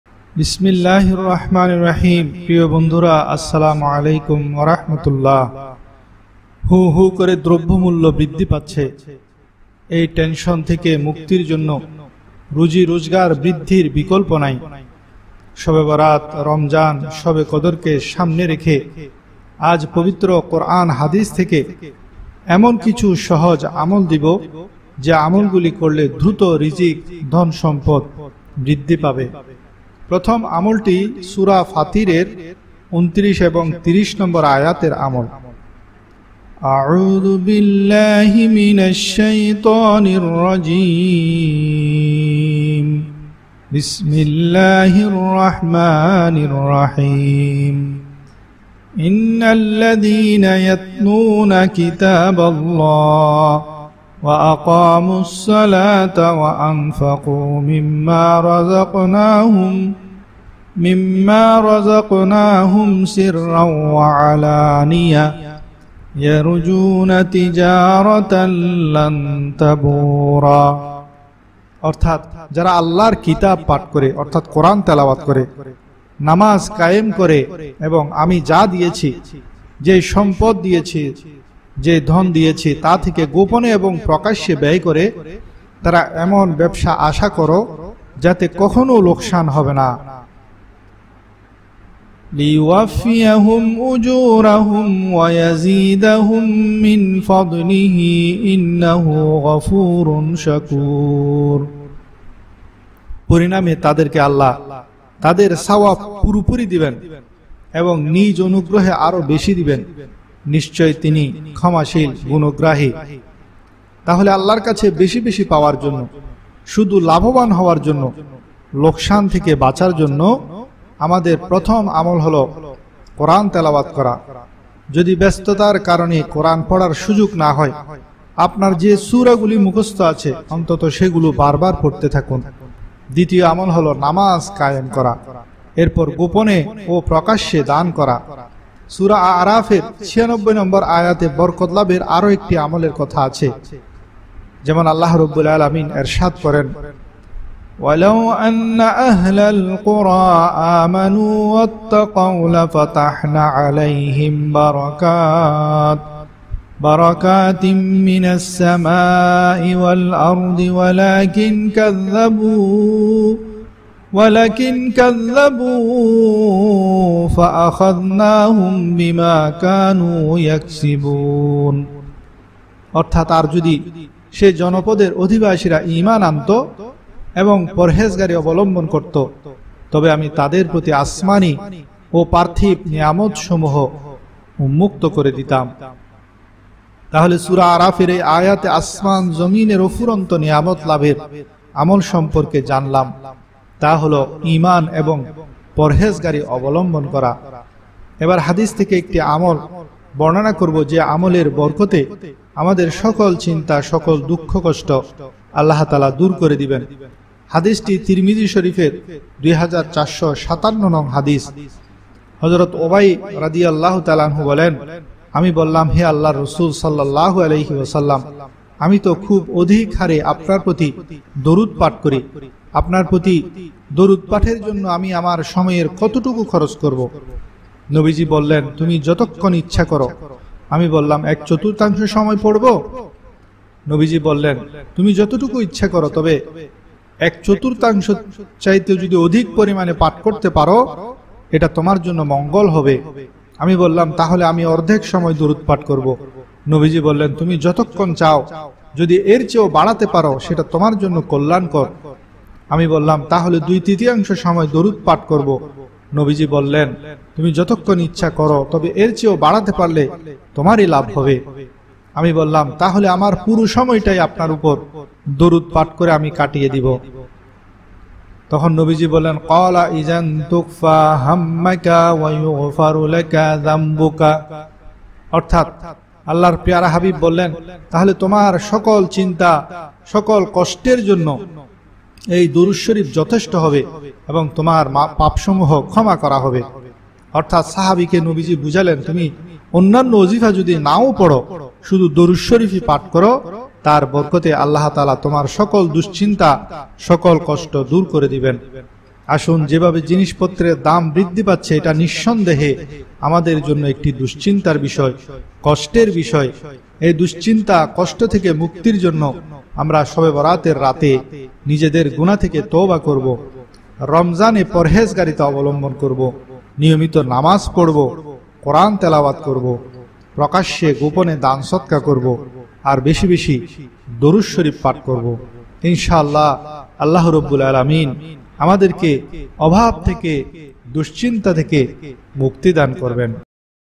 শবে বরাত রমজানে ধনী হওয়ার আমল। রিজিক বৃদ্ধির আমল। দুশ্চিন্তা দূর করার আমল। All bangla dua amol
প্রিয়বন্ধুরা
আগামী শবে বরাত শবে কদর ও রমজানে রিজিক বৃদ্ধি, আয় রুজি রোজগারে বরকত ধন সম্পদে বরকত দুশ্চিন্তা দূর করার কয়েকটি কোরআন হাদিসের আমল এই দ্রব্যমূল্যের উর্দ্ধগতিতে আপনাদের কাজ দিবে।